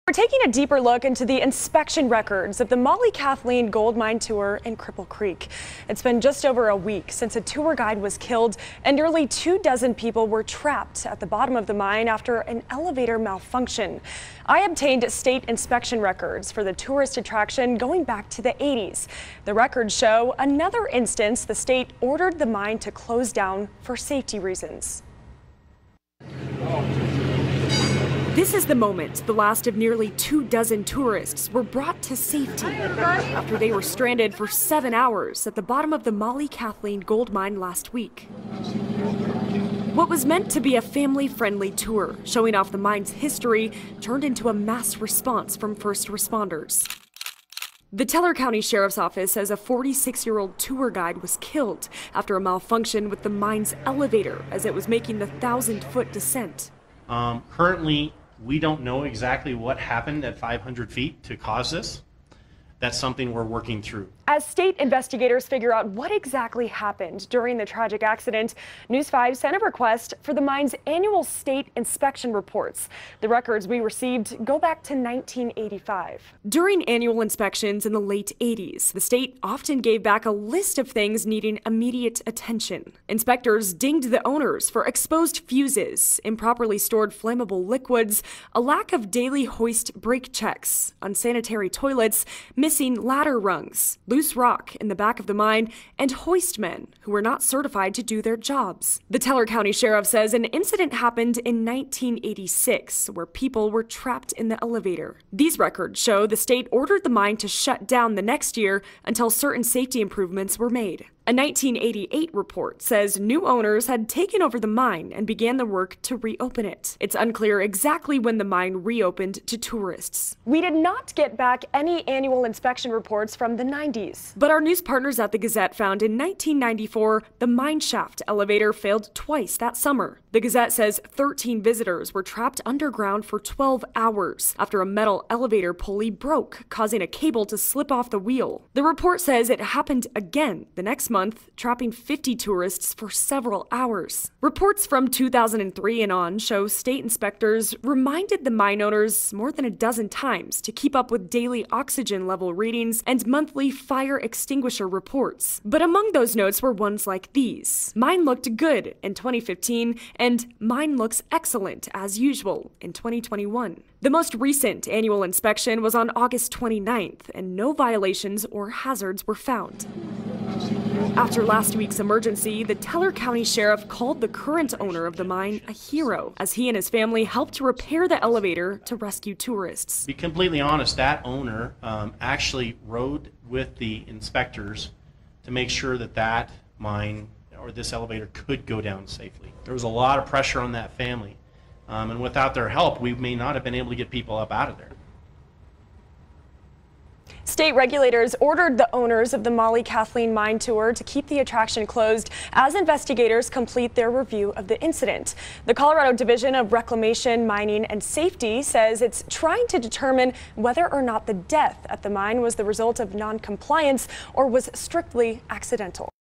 Cripple Creek gold mine inspection records show no recent violations
News5 obtained decades of annual inspection reports for the Mollie Kathleen Gold Mine Tour in Cripple Creek a week after a tour guide was killed when a mine shaft elevator malfunctioned.